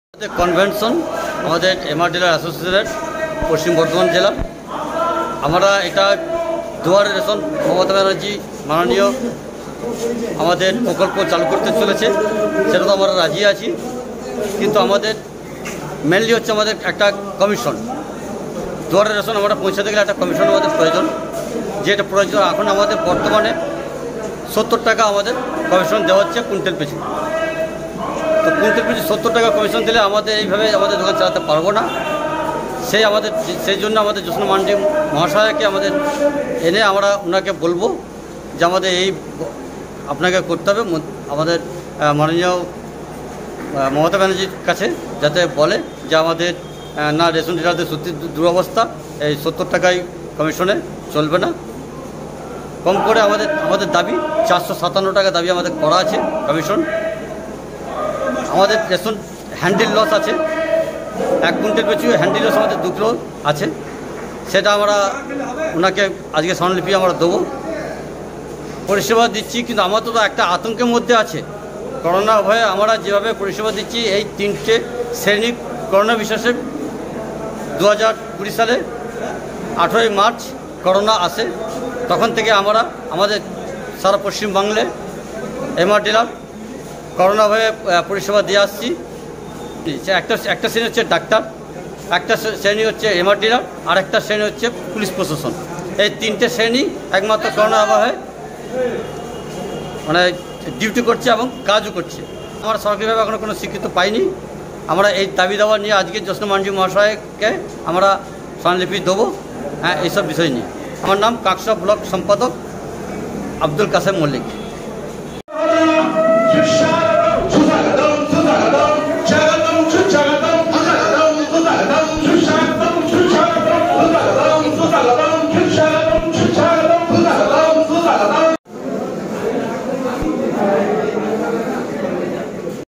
দুয়ারে রেশন কর্মসুচী সফল করতে হলে কমিশন বাড়াতে হবে রেশন ডিলারদের দাবী গনকনভেনশনে
মহুয়া ঘোষাল দুর্গাপুর ১৪ আগস্ট ২০২১
একগুচ্ছ দাবিকে সামনে রেখে রাজ্যব্যাপী রেশন ডিলার দের গন কনভেনশন অনুষ্ঠিত হলো কাঁকসা রথতলায় ।গণকনভেনশনে উপস্থিত ছিলেন পশ্চিম বাংলার খাদ্য ও সরবরাহ দপ্তরের প্রতিমন্ত্রী জ্যোৎস্না মান্ডি, জামুরিয়ার বিধায়ক হরেরাম সিং , গলসির বিধায়ক নেপাল ঘরুই , দুর্গাপুর পুর্বের বিধায়ক প্রদীপ মজুমদার ও শ্চিম বাংলার সমস্ত জেলার এম আর ডিলার অ্যাসোসিয়েশনের সদস্যরা । একগুচ্ছ দাবীর মধ্যে সবচেয়ে বোর দাবী প্রাপ্য কমিশনের বৃদ্ধি করতে হবে । রেশন ডিলারদের কথায় দুয়ারে রেশন প্রকল্প চালু করতে তাদের সমস্যা নেই , তারা রাজ্য সরকারের পাশেই আছেন কিন্তু দুয়ারে রেশন চালু করতে হলে তাদের দিতে হবে একটা কমিশন । এই মুহুর্তে রেশন ডিলাররা কুইন্টাল পিছু কমিশন পান ৭০ টাকা । কিন্তু রেশন ডিলারদের কথায় এই কমিশনে তারা রেশন ব্যবস্থা চালাতে পারবো না । রেশন ডিলাররা বলেন তাদের দাবি কমিশন ৪৫৭ টাকা করা । রেশন ডিলাররা খাদ্য  প্রতিমন্ত্রীর জ্যোৎস্না মাড্ডির কাছে একটি স্মারকলিপি দেন যাতে তাদের দাবী মুখ্যমন্ত্রীর কাছে পৌঁছায় । রেশন ডিলাররা জানাচ্ছেন তাদের হ্যান্ডেল  লস রয়েছে । এদের কথায় রেশন সামগ্রী আনা এবং রেশন দেওয়ার সময় বেশ কিছু সামগ্রীর ক্ষতি হয় । সেতাও বিবেচনা করতে হবে । এদিকে করোনার ভয়ঙ্কর সময়েও রেশন ডিলাররা রাজ্য সরকারের নির্দেশ মতো নিজেদের জীবনের ঝুঁকি নিয়েই কাজ করেছিলেন এবং এখনো করে যাচ্ছেন কিন্তু তারা তাদের প্রাপ্য স্বীকৃতি পান নি । রেশন ডিলাররা বলছেন এমন কিছু কারনে তাদের বিক্ষোভের মুখে পড়তে হচ্ছে যা তাদের গাফিলতির কারনে নয় ।
অনেক পরিবারের কর্তার আধার কার্ডে লিঙ্ক রয়েছে কিন্তু তিনি কর্মসুত্রে বাইরে থাকেন ফলে তার পরিবার রেশন তুলতে পারছেন ফলে সেইসব পরিবারের বিক্ষোভের মুখে পরটে হচ্ছে ডিলারদের । গ্রাহক রেশন ডিলার পরিবর্তনের আবেদন করলেও তাদের তা পরিবর্তন করা হচ্ছে না ফলে তাদের বিক্ষোভের মুখে পড়তে হচ্ছে ।
কার্যত রেশন ডিলারদের এখন এমন সব সমস্যার মুখে পরিস্থিতির মধ্যে পড়তে হচ্ছে যাতে তাদের কোন ভূমিকাই নেই ।